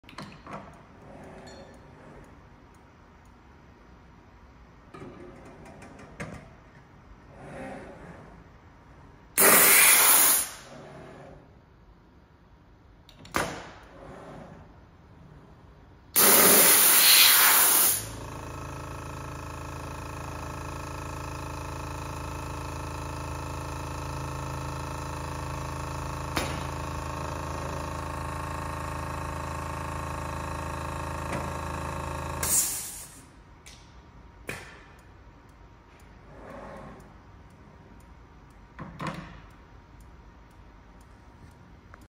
Robot Machine Tending Simulation
Simulating tending a CNC mill with Robotiq’s UR Machine Tending Solution.